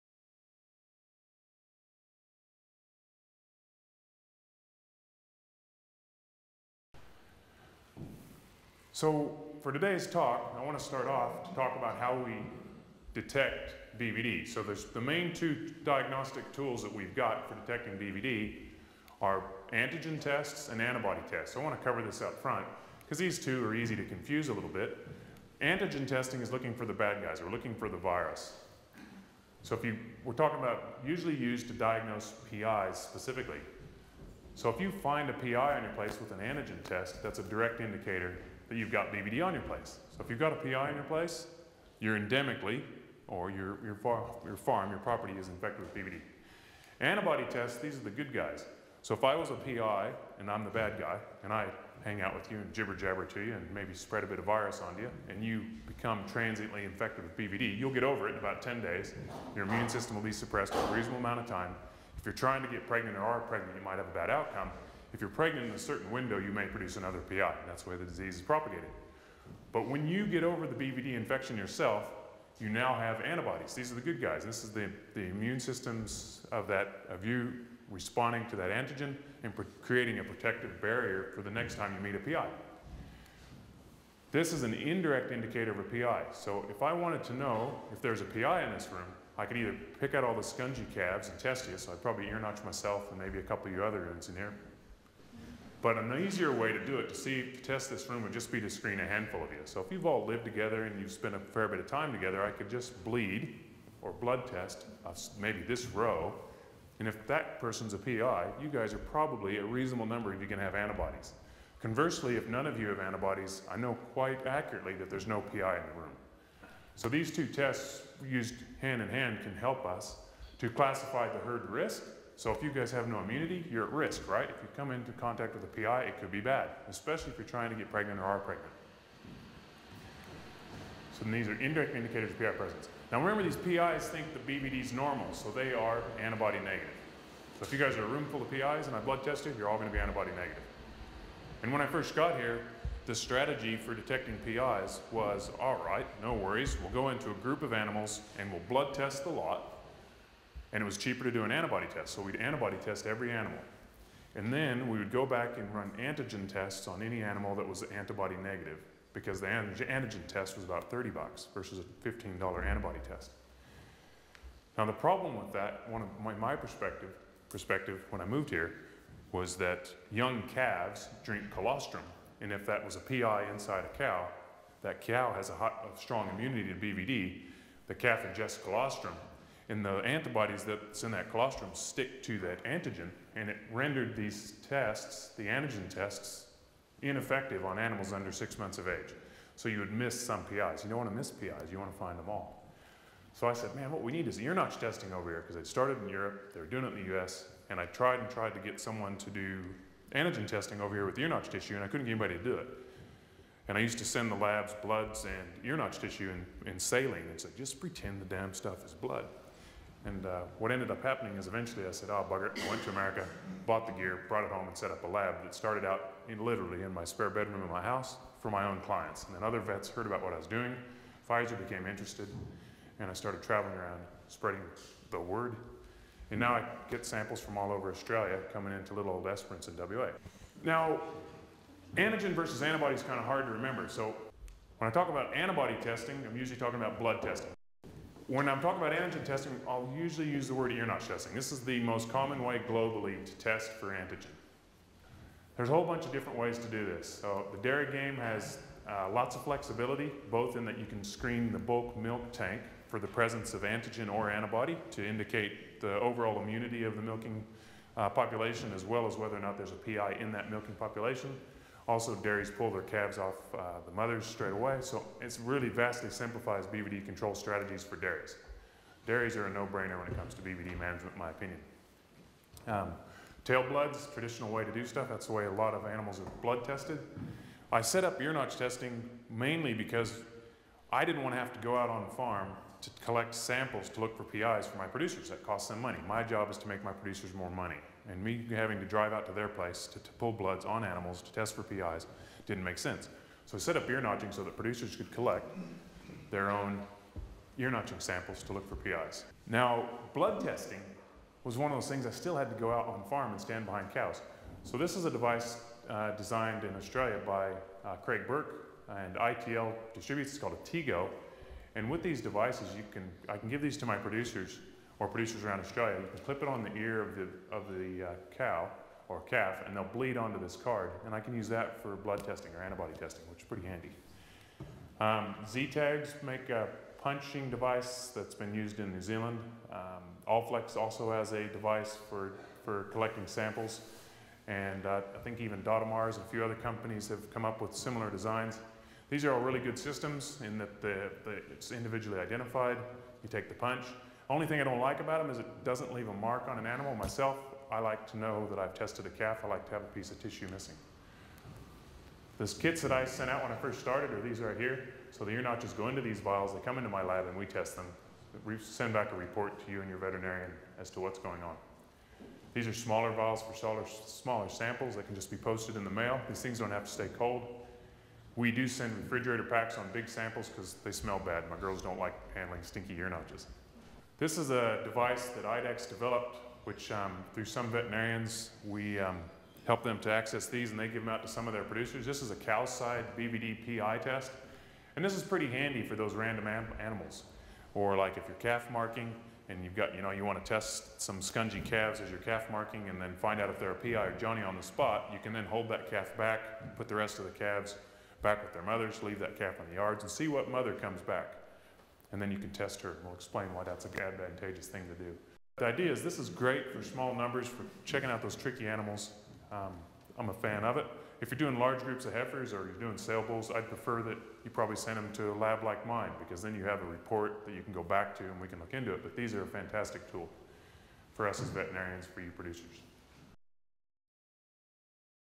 Chapter 2: BVDV: Diagnostic Tools
Dr. Bergman, DVM discusses how to diagnose BVDV infection and the differences between diagnostic tools.